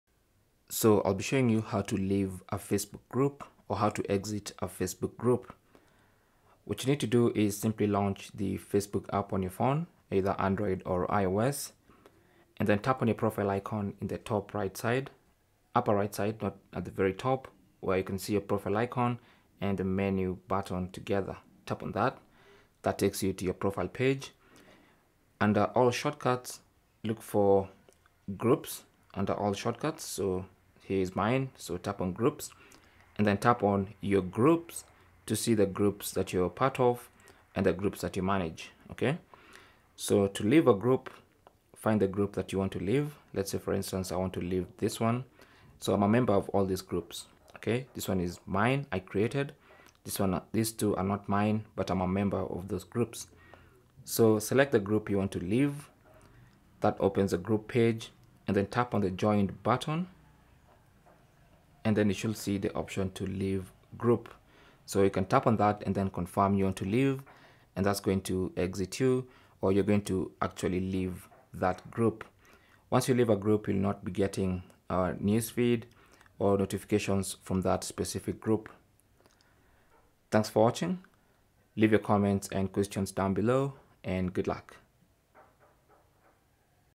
How To Leave / Exit a Facebook Group On Mobile
Here's how to leave or exit a Facebook group.
You might have joined many Facebook groups and would like to exit the ones you aren't an active member of. Or maybe you'd like to get out of any social groups that you're in.

Facebook allows you to leave any groups you've entered at any given time. You can exit from the Facebook application as shown in this tutorial video.

How To Exit A Facebook Group On Mobile
Turn on your phone
Launch the Facebook application
Tap on your profile icon at the upper right of the screen
Tap on Groups under All Shortcuts subheading. Here you'll be able to see all the Facebook groups that you're in
Tap on Your Groups
Select the group you'd like to exit
On the group's page, tap on Joined
Tap on Leave Group from the pop-up menu that appears and tap on Leave Group to confirm the changes

Cell Phone Tripod Adapter